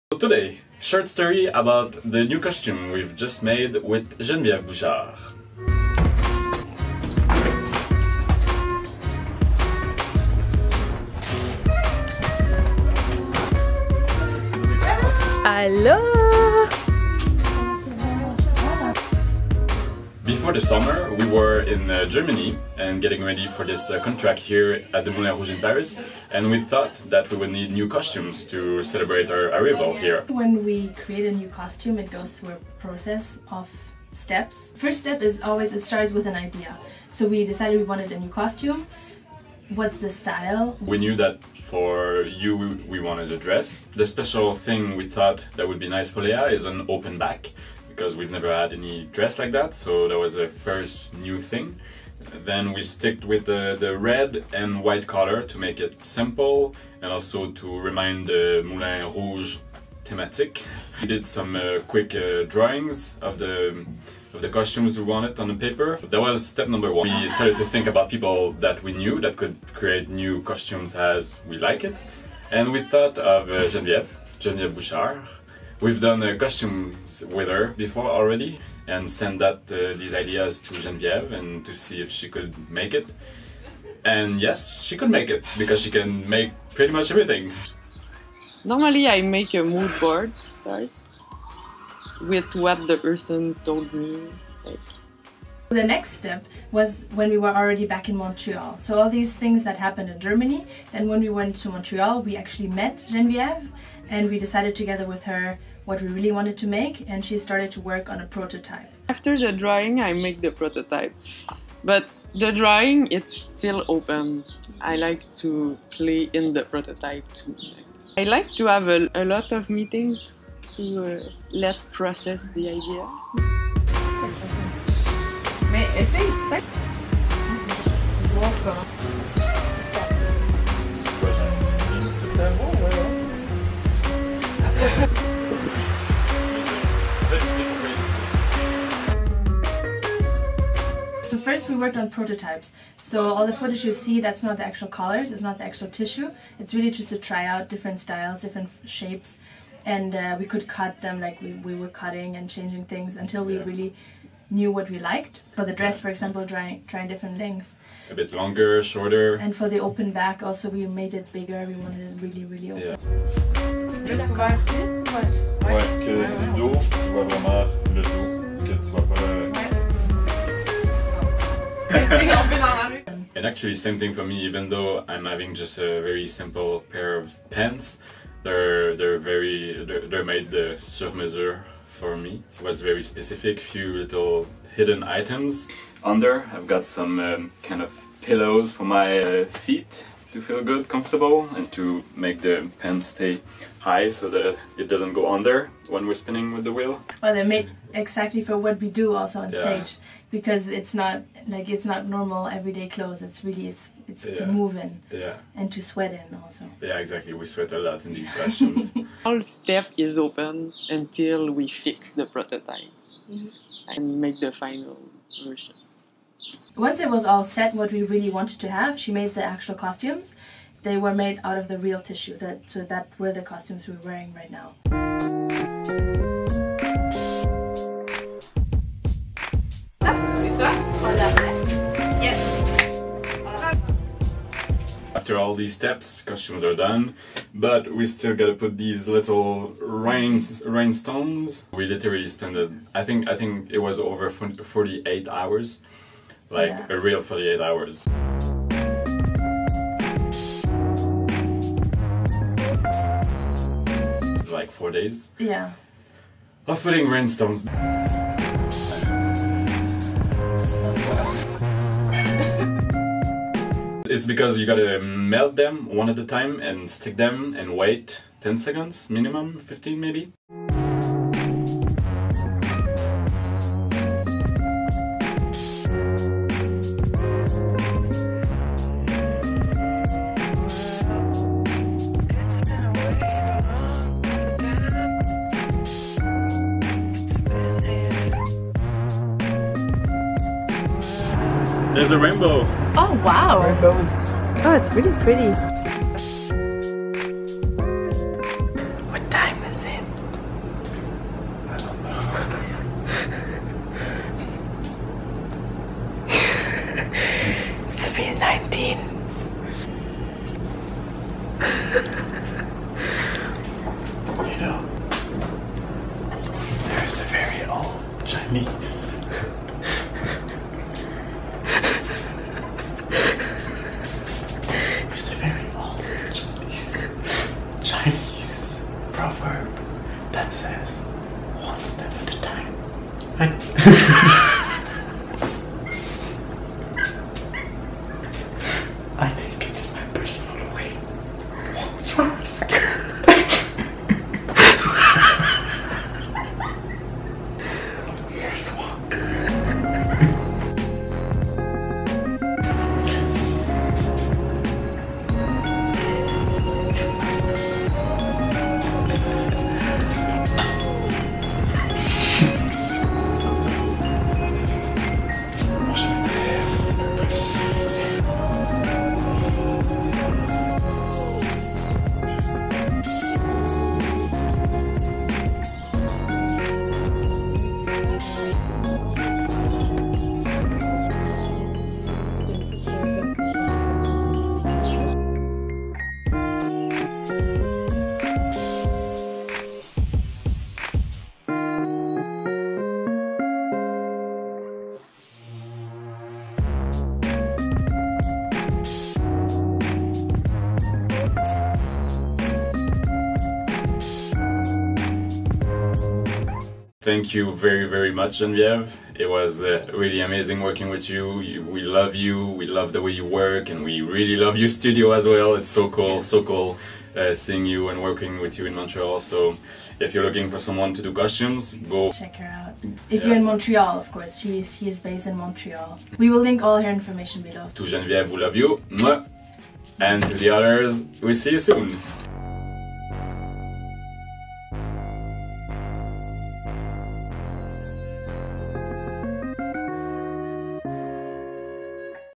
New Costumes By Geneviève Bouchard
We have new costumes! Follow us through the whole process of making new costumes ;)

CONCEPT & EDITING: Lea Toran Jenner
FILMING:  Lea Toran Jenner & Francis Perreault

Lea & Francis,
Duo Unity